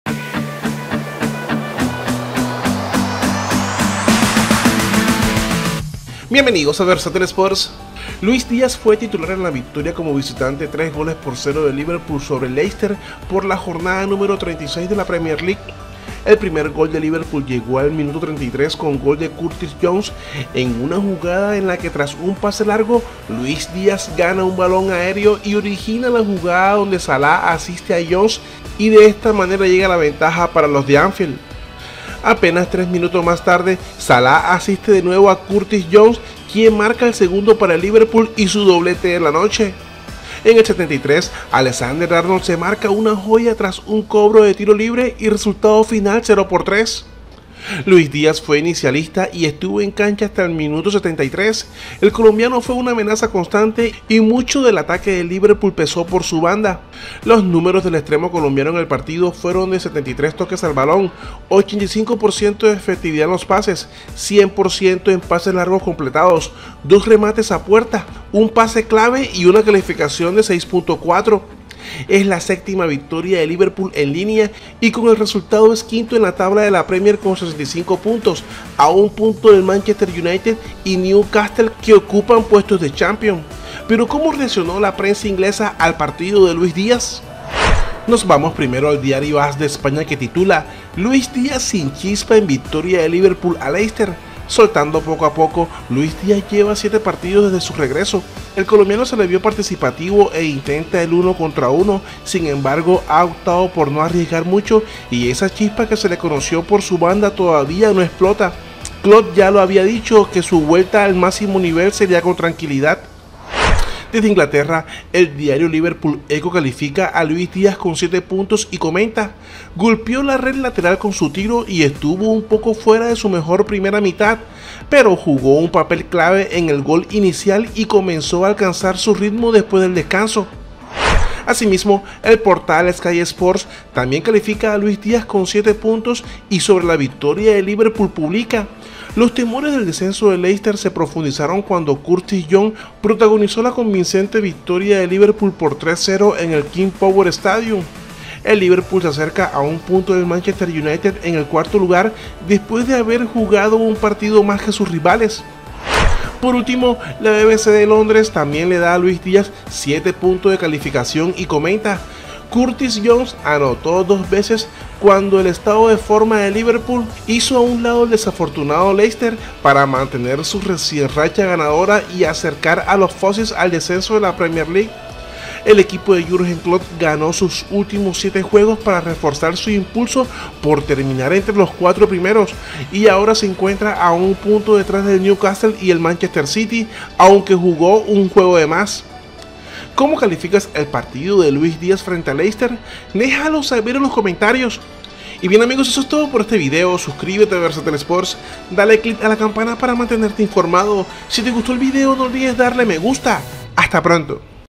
ASI REACCIONA PRENSA INGLESA a PARTIDO de LUIS DIAZ LEICESTER vs LIVERPOOL
Luis Diaz fue titular en la victoria como visitante 3-0 de Liverpool sobre Leicester por la jornada numero 36 de la Premier League.
El primer gol de Liverpool llegó al minuto 33 con gol de Curtis Jones, en una jugada en la que tras un pase largo Luis Díaz ganó un balón aéreo y origina la jugada donde Salah asiste a Jones y llega la ventaja para los de Anfield.
Apenas tres minutos mas tarde, Salah asiste de nuevo a Curtis Jones quien marca el segundo para Liverpool y su doblete de la noche.
En el 73 Alexander Arnold se marca una joya tras un cobro de tiro libre y resultado final 0-3.
Luis Díaz fue inicialista y estuvo en cancha hasta el minuto 73.
El colombiano fue una amenaza constante y mucho del ataque del Liverpool pesó por su banda.
Los números de Luis Díaz en el partido fueron 73 toques al balón, 85 % de efectividad en los pases, 100 % en pases largos completados, dos remates a puerta, un pase clave y una calificación de 6.4.
Es la séptima victoria Liverpool en línea y con el resultado es quinto en la tabla con 65 puntos a 1 un pinto del Manchester united y Newcastle que ocupan puestos de champions.
Pero cómo reaccionó la prensa inglesa al partido de Luis Díaz.